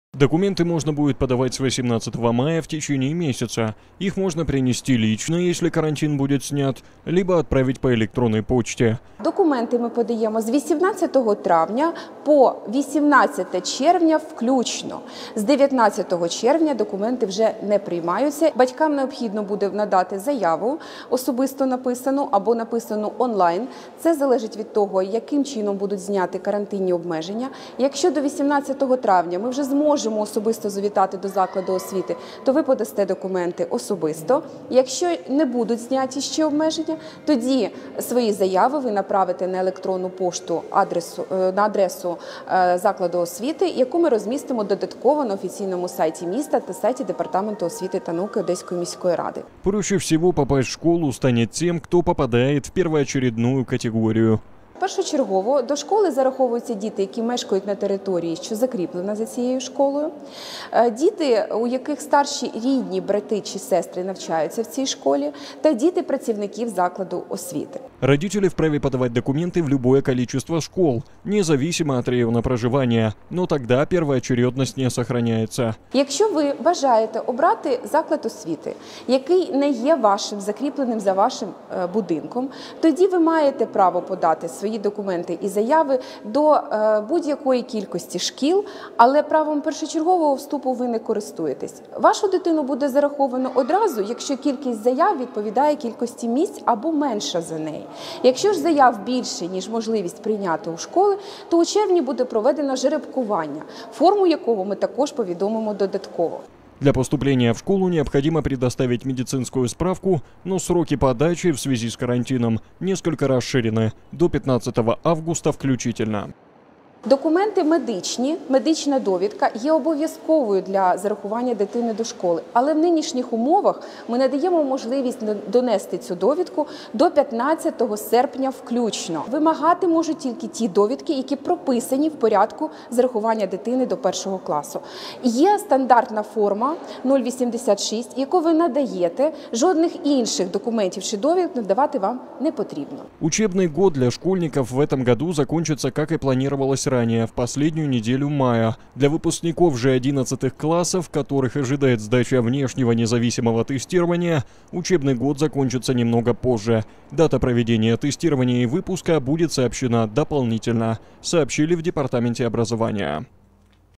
В Одессе изменен порядок зачисления первоклашек
Документы для поступлення в первуй класс можно подавать с 18 мая в течении месяца. Их можно принести лично, если карантин будет снят, либо отправить по электронной почте.

Елена Буйневич, директора Департамента образования и науки Одесского городского совета
Документи ми подаємо з 18 травня до 18 червня включно, з 19 червня документи вже не приймаються. Батькам необхідно буде подати заяву, особисто написану, або онлайн, це залежить від того, коли будуть зняті карантинні обмеження, якщо до 18 травня ми вже зможемо особисто завітати до закладу освіти, то зможете подати документи особисто, якщо не будуть, тоді свої заяви ви направите на електрону пошту на адресу закладу освіти, яку ми розмістимо додатково на сайті міста а також сайті департаменту.

Проще всего попасть в школу станет тем, кто попадает в первоочередную категорию

Першочергово до школи приймаються діти що мешкають на території, що закріплені за цією школою, діти у яких старші рідні, брати чи сестри навчаються в цій школі, та діти працівників закладів освіти.

Родители вправе подавать документы в любое количество школ, не зависимо от района проживания, но тогда первоочередность не сохраняется.

Якщо ви бажаєте обрати заклад освіти, який не є вашим,закріплений за вашим будинком, тоді ви маєте право подати свої документи і заяви до будь якої кількості шкіл, але правом першочергового вступу ви не користуєтесь, вашу дитину буде зараховано одразу, якщо кількість заяв відповідає кількості місць, або менше за нею, якщо заяв буде більше ніж можливість прийняти у школу, то у червні буде проведено жеребкування форму якого ми також повідомимо додатково.

Для поступления в школу необходимо предоставить медицинскую справку, но сроки подачи в связи с карантином несколько расширены, до 15 августа включительно.

Документи медичні, медична довідка є обов’язковою до зарахування до школи, але в нинішніх умовах, ми надаємо можливість донести цю довідку до 15 серпня включно вимагати можуть тільки ті довідки, які прописані в порядку зарахування дитини до першого класу є стандартна форма 086 яку ви надаєте, жодних інших документів чи довідок надавати вам непотрідно

Учебный год для школьников в этом году закончится, как и планировалось ранее, в последнюю неделю мая, для выпускников же 11 классов, которых ожидает сдача внешнего независимого тестирования, учебный год закончится немного позже. Дата проведения тестирования и выпуска будет сообщена дополнительно.